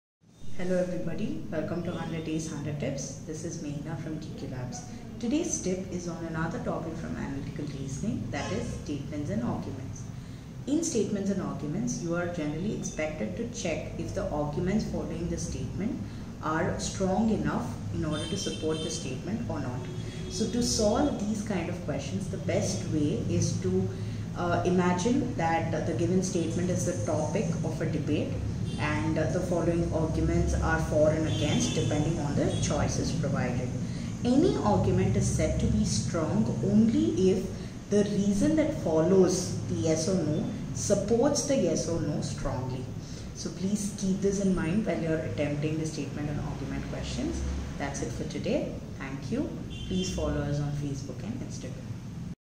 Tip 21 Statements & Arguments
Hope you liked the tips that we shared with you so far. Today's tip is on handling questions related to Statements & Arguments.

In Statements and Arguments, it is ideal that you check if the arguments following the statements are strong enough in order to support the statement or not.

The best way to tackle these types of questions are to imagine that the given statement is a topic of debate and the arguments following the statement are “for” or “against” that particular statement. Use the answer choices as a guideline to decide this.

Any argument is said to be strong only if the reason that follows ‘yes or no’ supports the ‘yes or no’ strongly. Keep the above points in mind while answering the questions related to Statements and Arguments.

Follow DQ Labs for such interesting tips